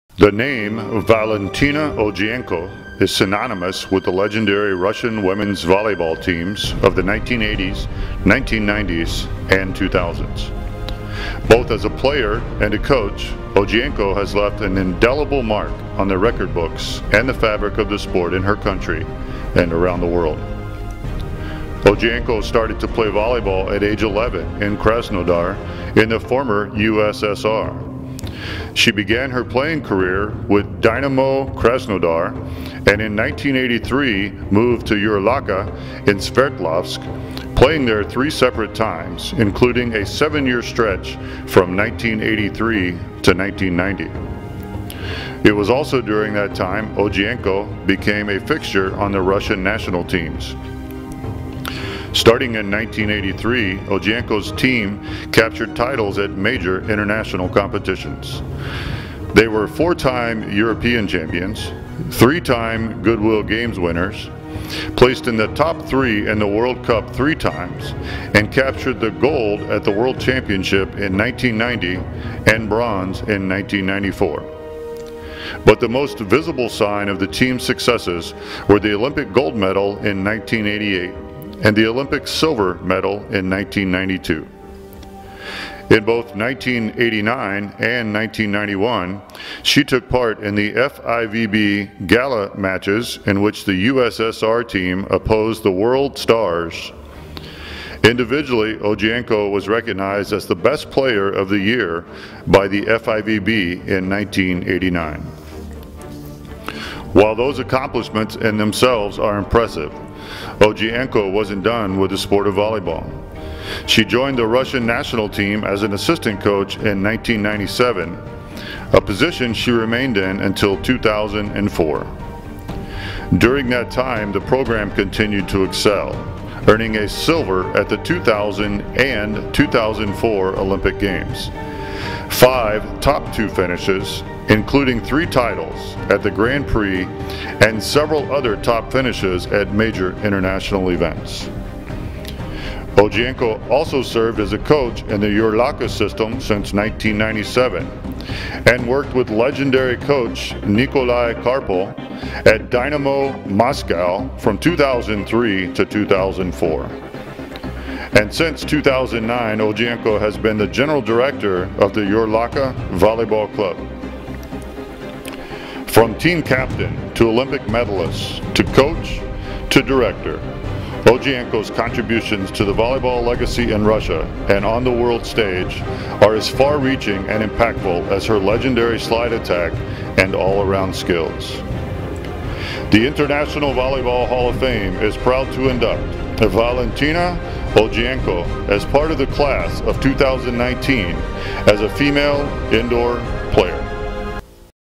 Valentina Ogienko (Russia) - 2019 International Volleyball Hall of Fame Inductee Introduction Video
The name Valentina Ogienko is synonymous with the legendary Russian women’s volleyball teams of the 1980s, 1990s and 2000s. Both as a player and a coach, Ogienko has left an indelible mark on the record books and the fabric of the sport in her country and around the world. IVHF​